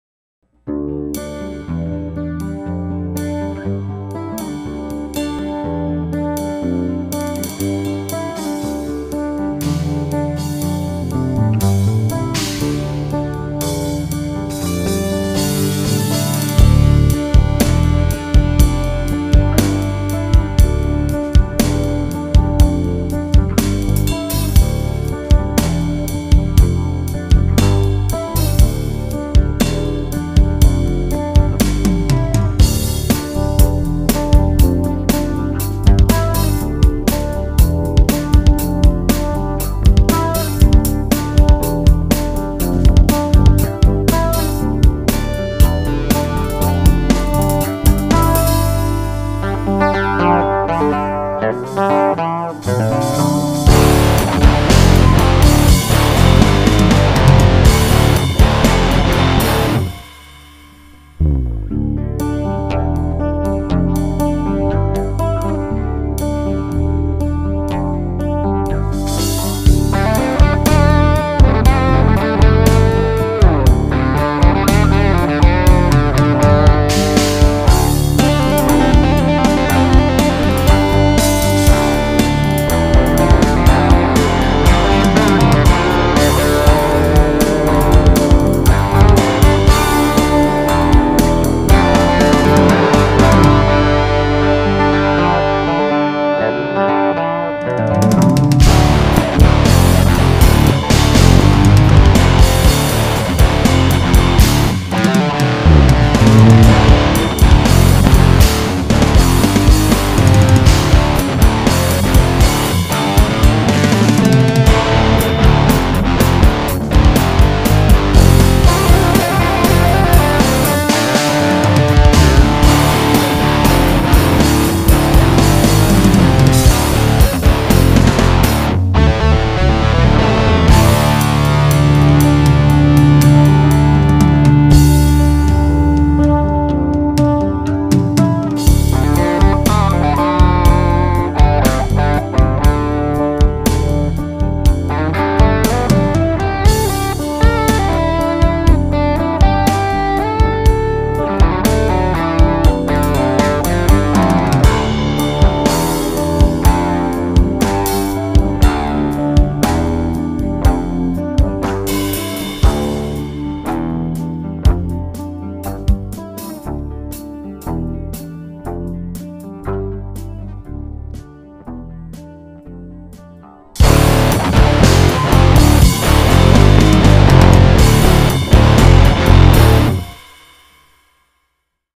JAMMING OUT
RECORDING A TUNE I HAD RUNNING IN MY HEAD ALL DAY.